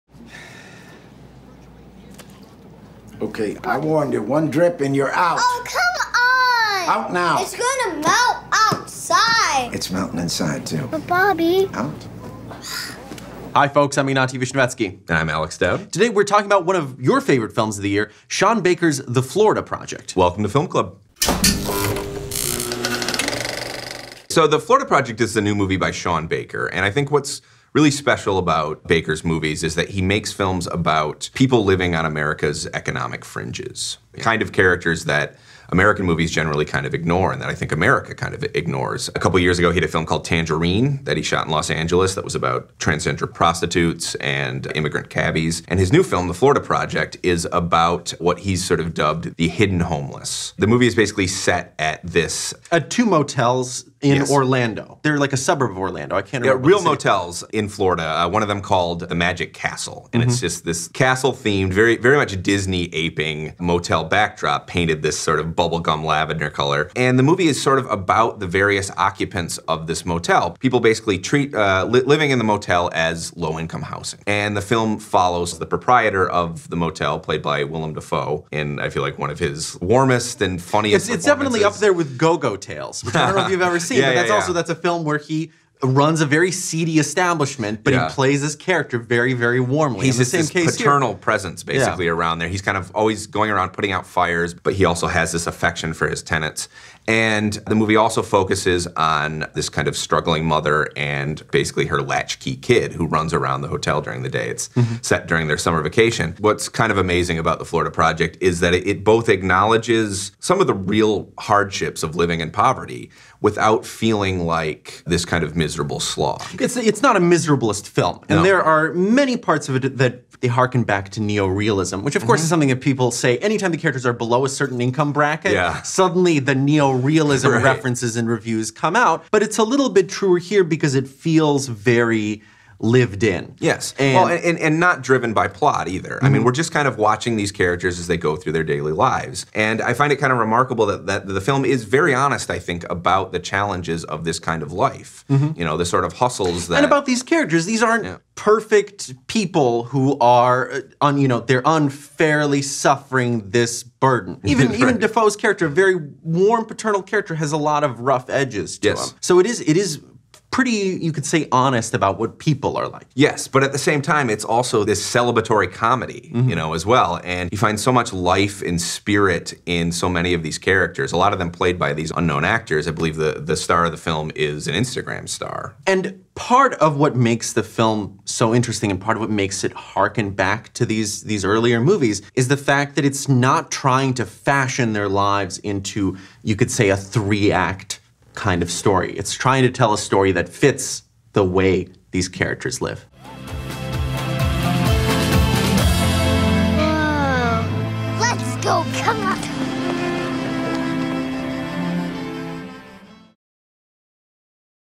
The Florida Project | Discussion & Review | Film Club
For his follow-up to the riotous underground comedy Tangerine, writer-director Sean Baker heads south to the Sunshine State, where he turns his camera on the occupants of a purple-painted motel called The Magic Castle. The Florida Project, which starts trickling into theaters tomorrow, traces the ups and downs of the establishment’s cast of impoverished characters, including the tough-love manager played by a superbly funny Willem Dafoe. On this episode of Film Club, A.A. Dowd and Ignatiy Vishnevetsky discuss why Baker’s moving, uproarious slice of life may be one of the best movies of the year.